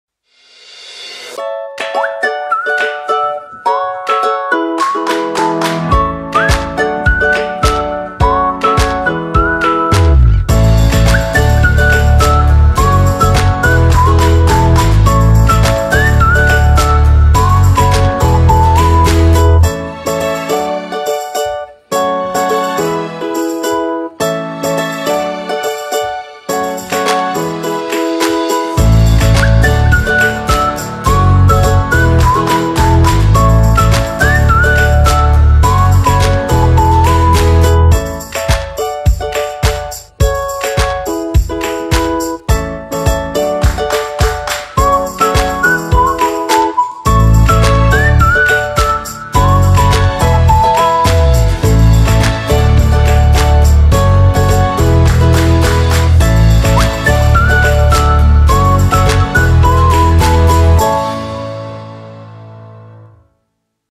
Outfits with Maliah!
Maliah loves showing you her favorite cozy sweaters and jeans. Let's see what they look like on you!